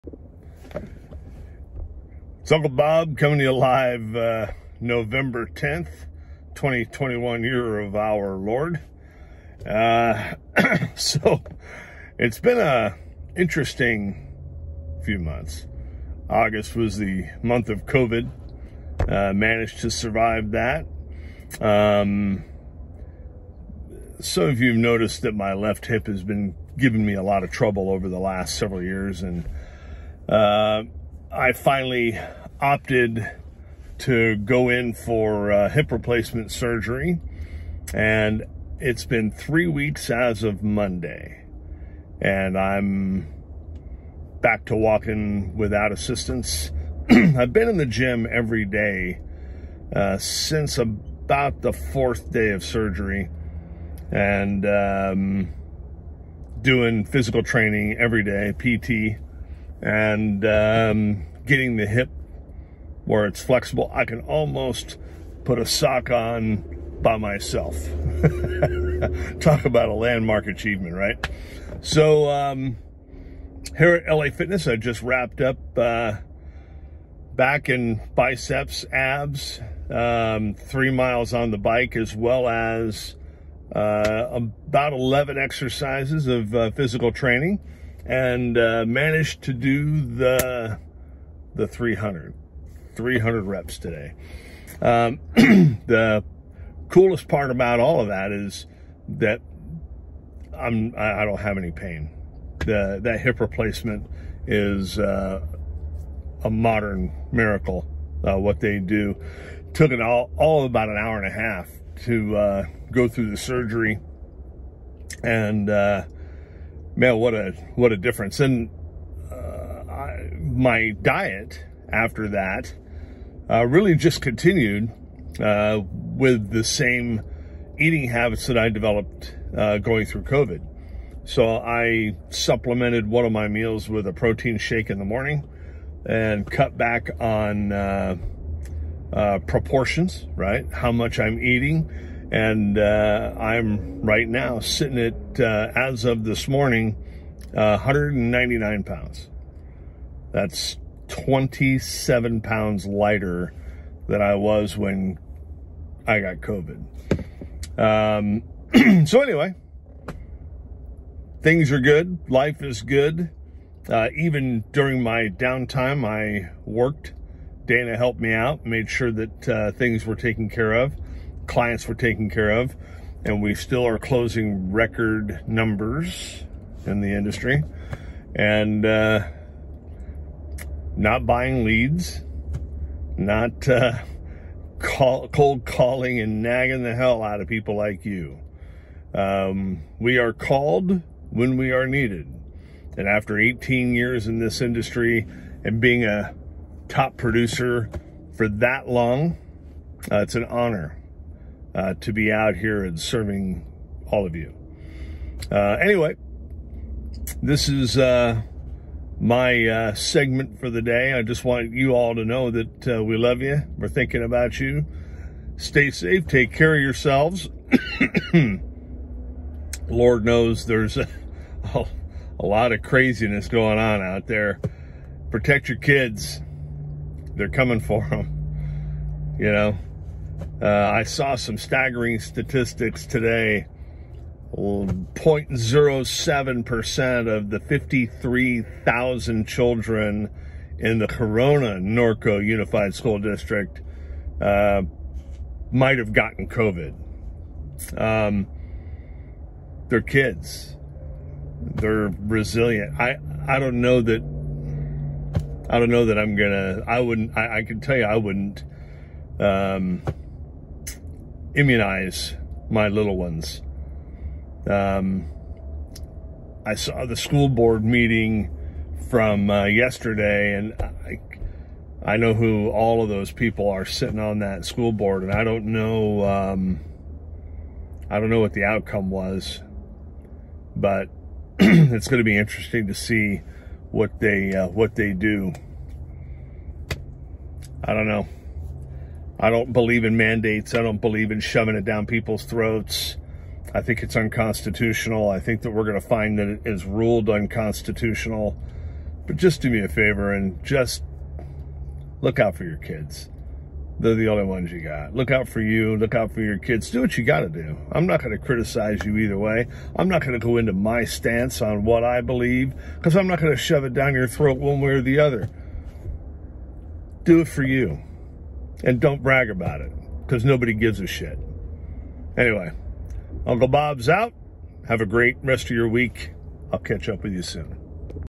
The Bionic Hip
Uncle Bob’s VLOG-11/10/2021-the Bionic Hip!  Take care of you, take care of your kids and by God, don’t be paralyzed by fear!